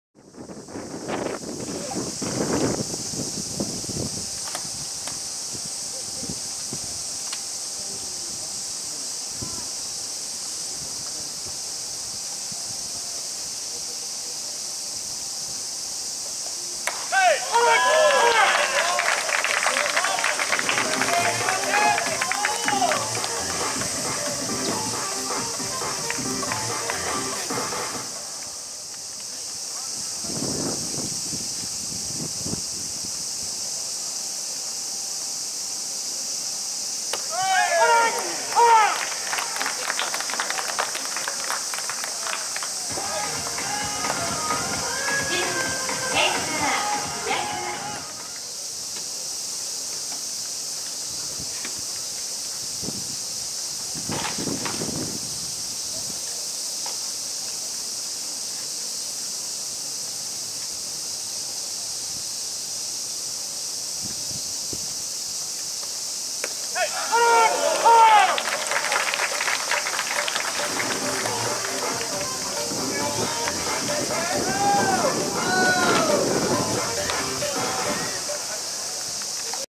【見逃し三振】古賀球審 2020/08/11
イースタンリーグ　鎌ヶ谷スタジアム
日本ハム対巨人
重信選手　6回　吉川投手
上野選手　7回　藤岡投手
加藤壮選手　8回表　鈴木健矢投手